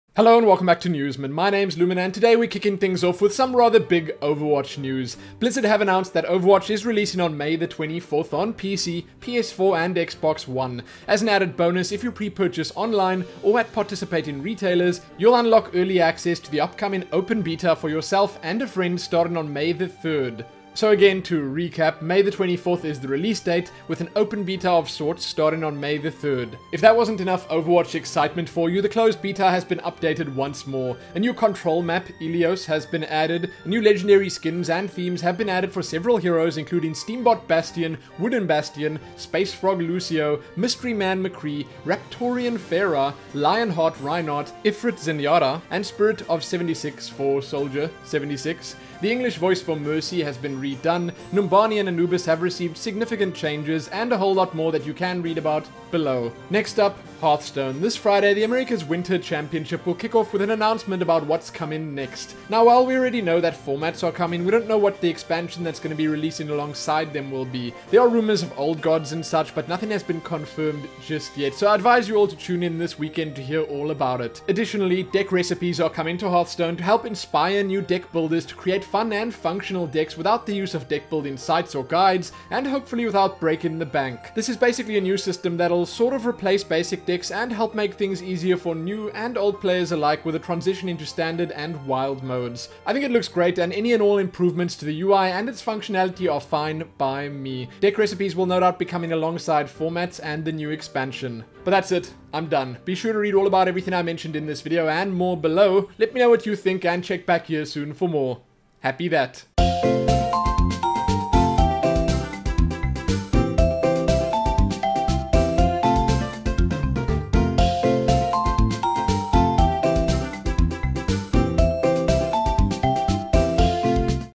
Overwatch Release Date & Open Beta, Hearthstone Deck Recipes & More! (Newsmin 09/03/16)
In this episode of Newsmin we're looking at the announcement of Overwatch's release date and open beta, an upcoming Hearthstone announcement, Hearthstone Deck Recipes and more!

► Overwatch

► Hearthstone

Let’s Get Brewing with Deck Recipes!

► Diablo III

► Lumin Links ◄